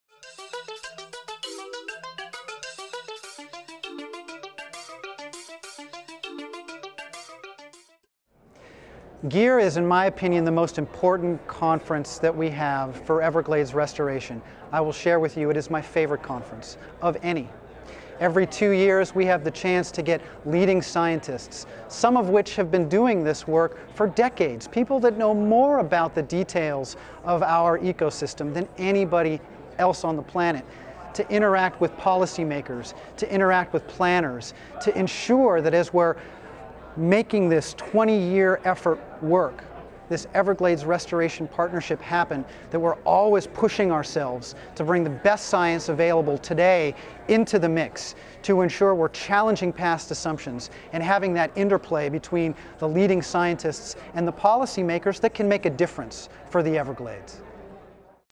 Paul Souza - formerly with US Fish & Wildlife - talks about the GEER Science Conference
Paul talks about the benefits of attending the Greater Everglades Ecosystem Restoration Science Conference (GEER).